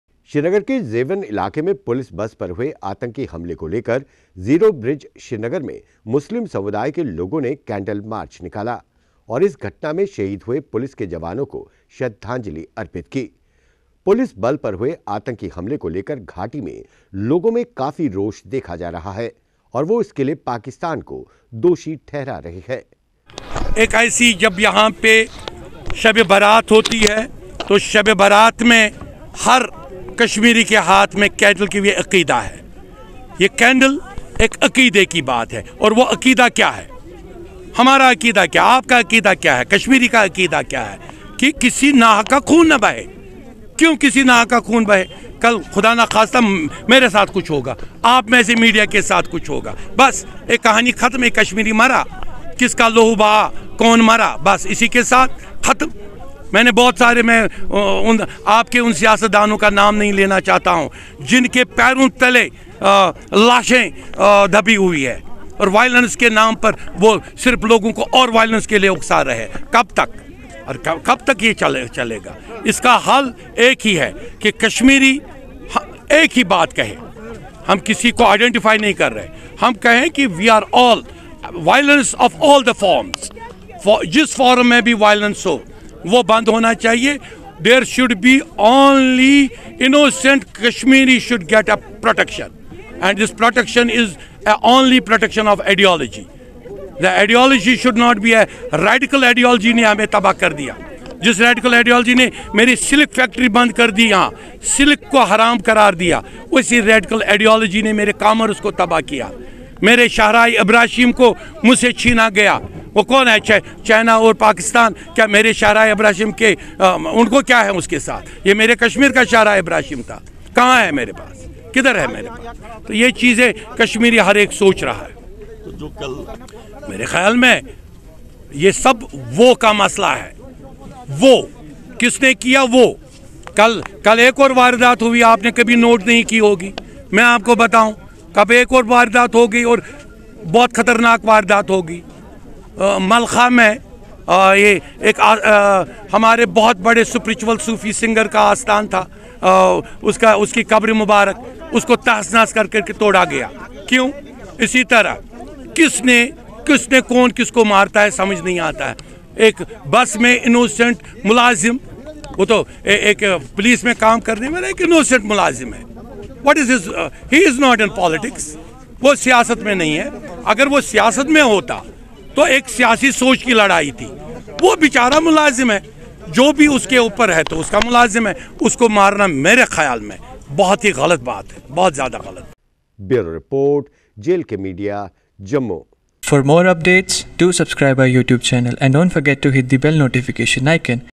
JLKMEDIA श्रीनगर के zewan इलाके में पुलिस बस पर हुए आतंकी हमले को लेकर कैंडल मार्च
JLKMEDIA श्रीनगर के zewan इलाके में पुलिस बस पर हुए आतंकी हमले को लेकर zero bridge श्रीनगर में मुस्लिम समुदाय के लोगों ने कैंडल मार्च निकाला और इस घटना में शहीद हुए पुलिस के जवानों को श्रद्धांजलि अर्पित की। पुलिस बस पर हुए आतंकी हमले को लेकर घाटी के लोगों में खासा रोष है और वह इसके लिए पाकिस्तान को दोषी ठहरा रहे हैं
Video Editor :- Kuldeep Raj